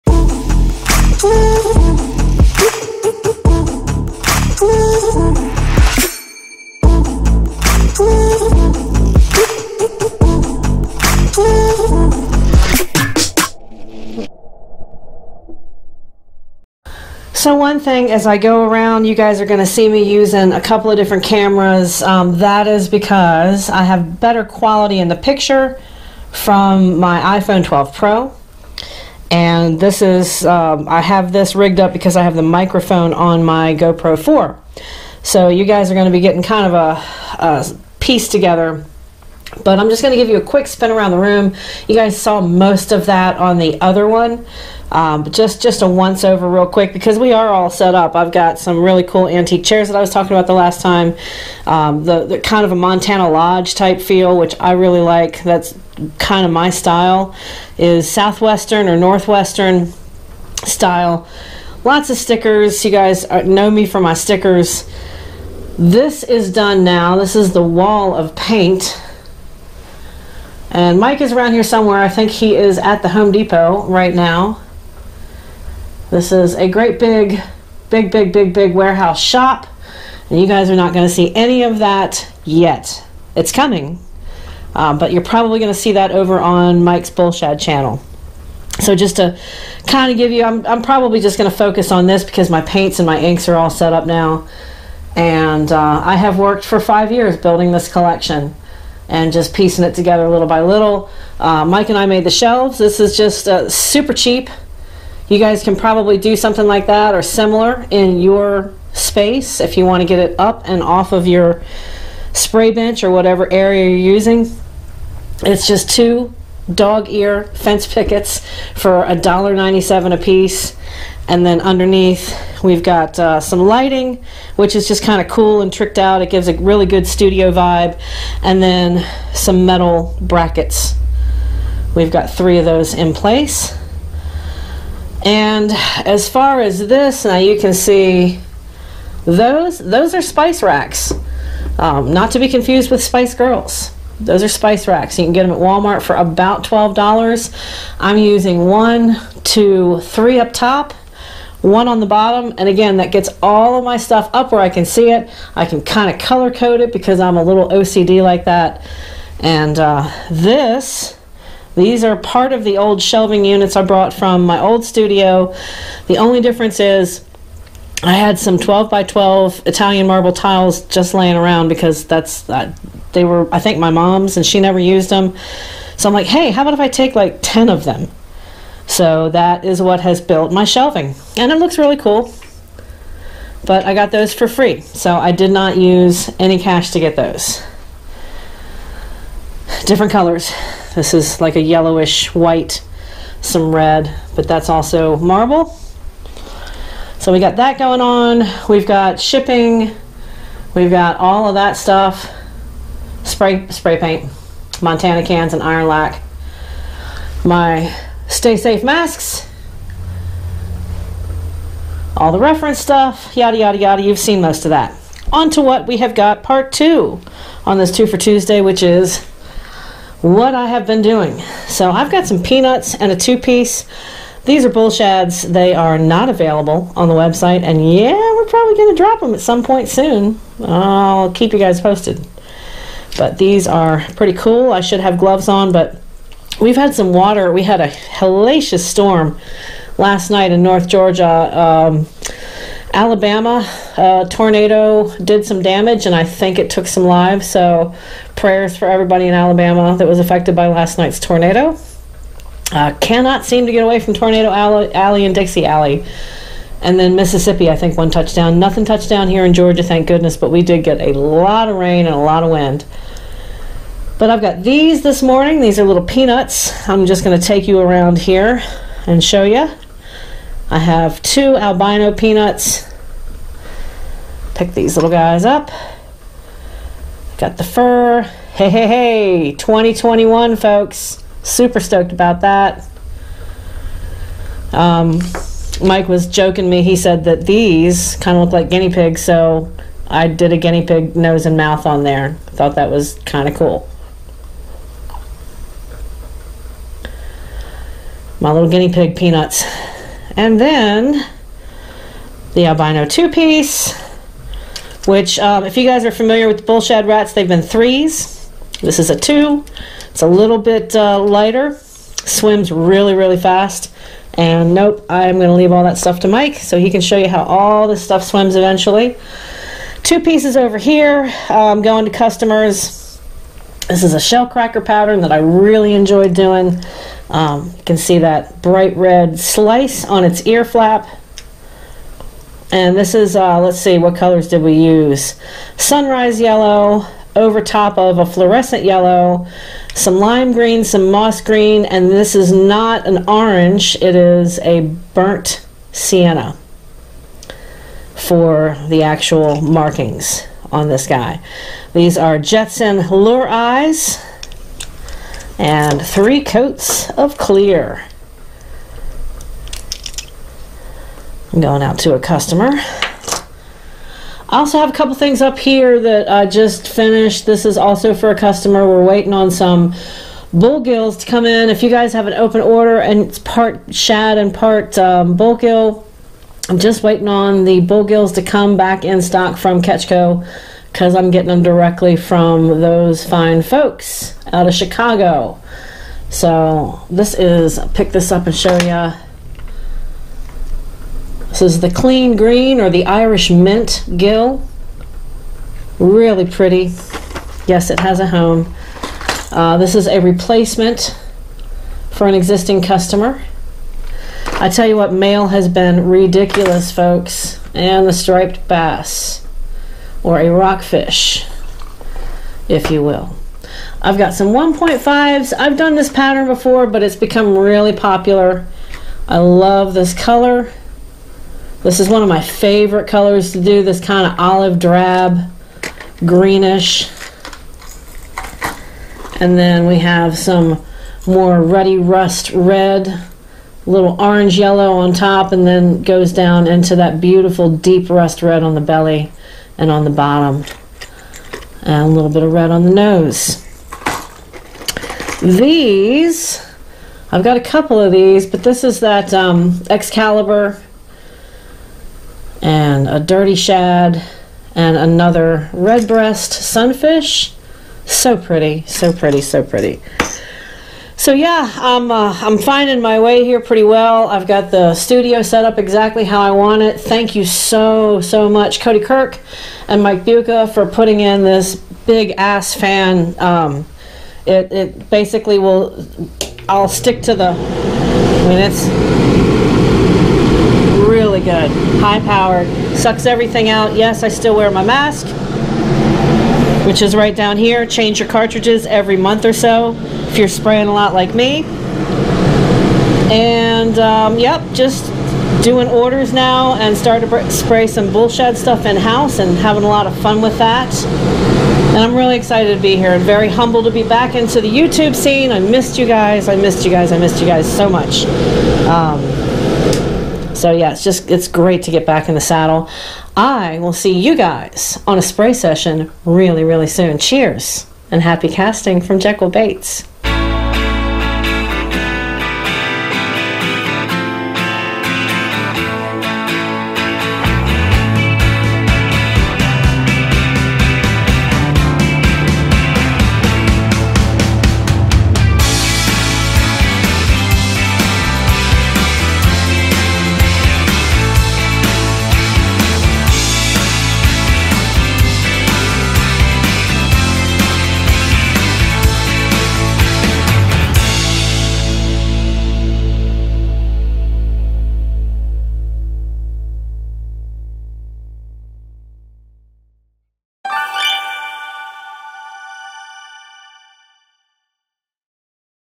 2021 SHOP UPDATE (and show and tell... lol)
I'm baaaaaaaaccckk!!
Miss y'all so much!! Here's a first ACTUAL shop update, complete with pieces, the finished studio and yes... even a guinea pig. Cheers, and thanks for stopping by the channel. Great to see you again!
Jen at JBCo.

MYSTERY TACKLE BOX...BECAUSE YES PLEASE!!!

Respirators:
3M (Large)
3M (Small)